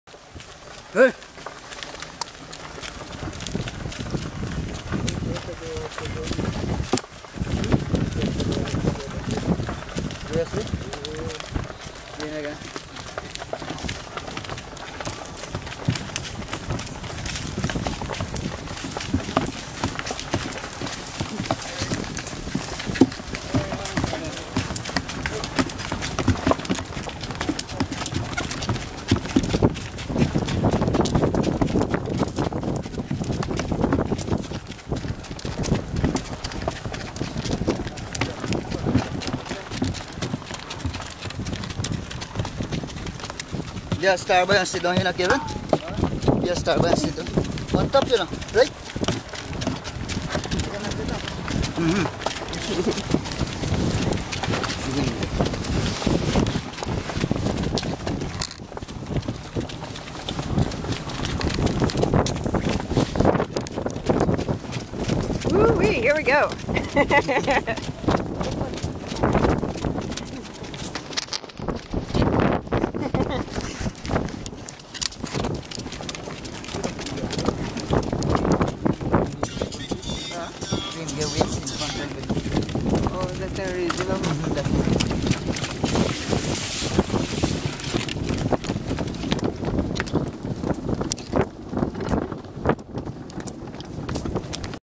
Ox Cart Ride Into the Jungle
Easy part of the ox cart ride into jungle from Nappi Village to Maipaima Lodge, Guyana.